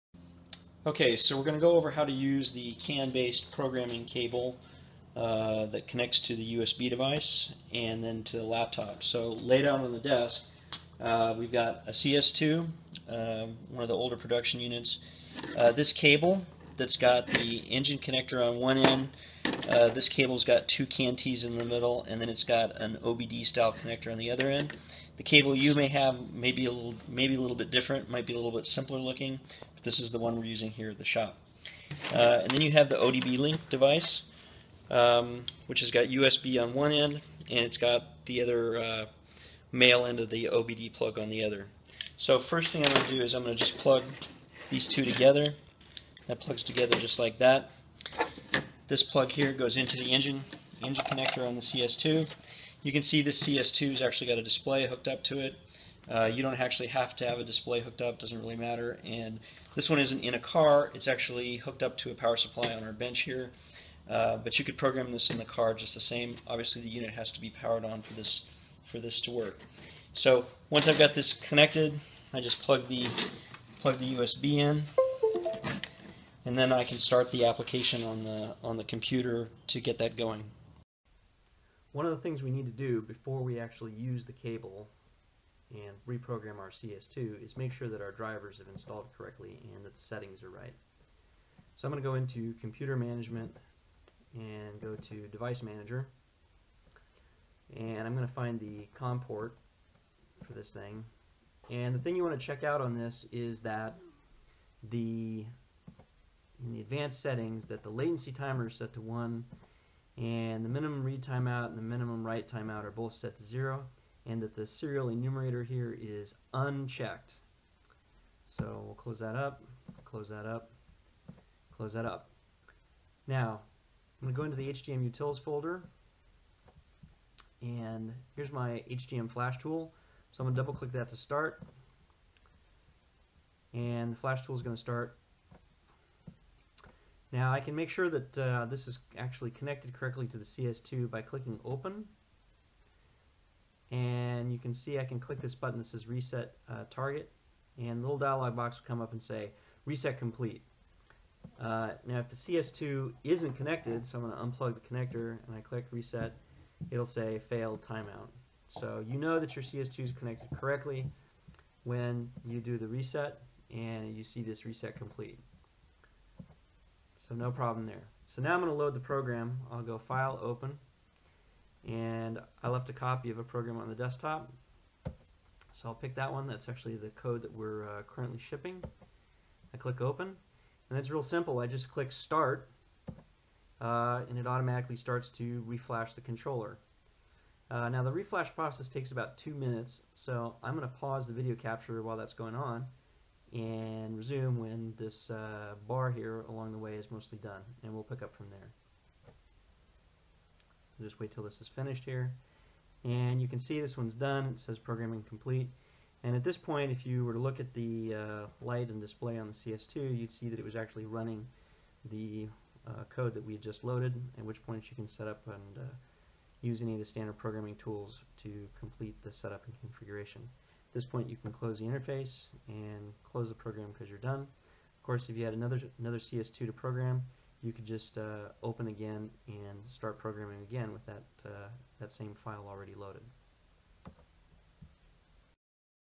Using The HGM Flash Tool
HGM Flash tool is used to program the legacy COMPUSHIFT II

*COMPUSHIFT II has been superseded by the COMPUSHFT 3 standalone transmission controller, an advanced controller that can be programmed via Bluetooth and the CSSetup application on your mobile device. HGM Flash tool is no longer available for download.